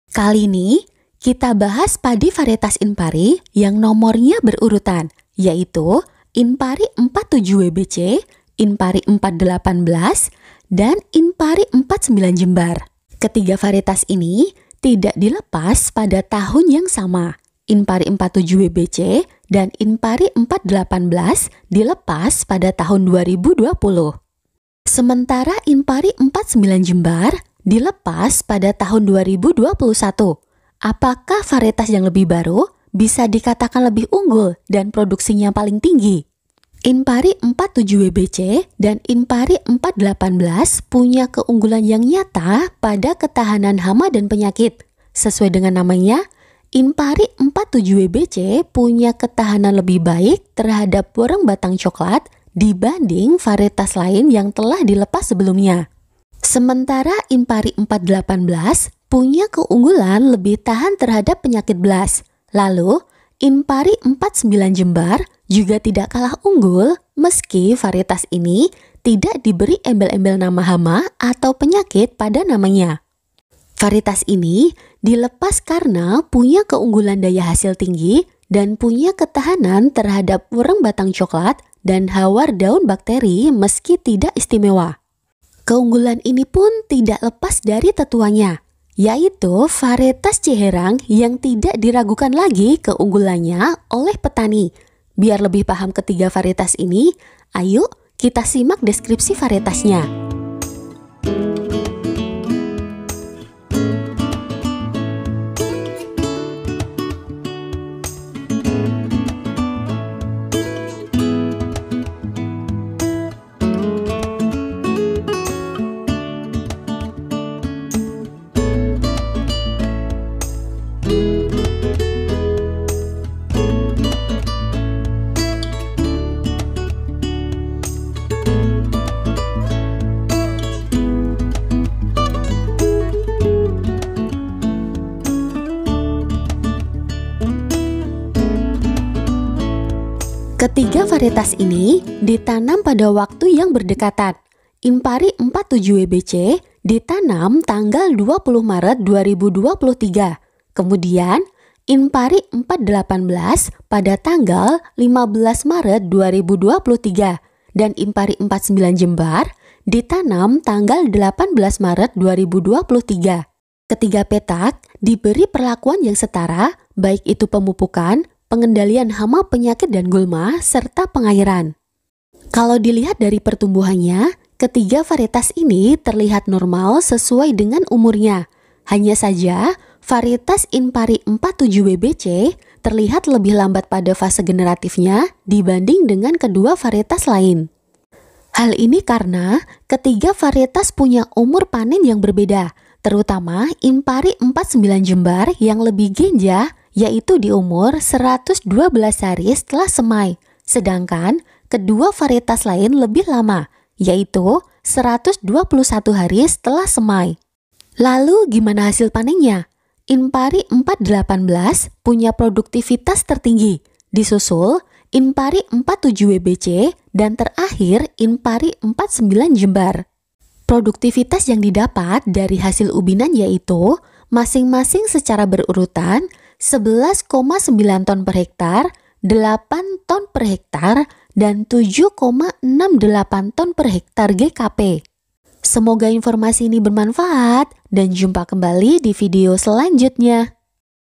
Inpari 47 WBC, Inpari 48 Blas dan Inpari 49 Jembar, Paling Baru Lebih Unggul?
Varietas Padi dengan Penamaan Inpari dengan nomor berurutan yaitu Inpari 47 WBC, Inpari 48 Blas dan Inpari 49 Jembar kita bandingkan ditanam pada waktu dan lokaso hamparan yang sama. Vagaimana hasilnya apakah varietas terbaru lebih unggul?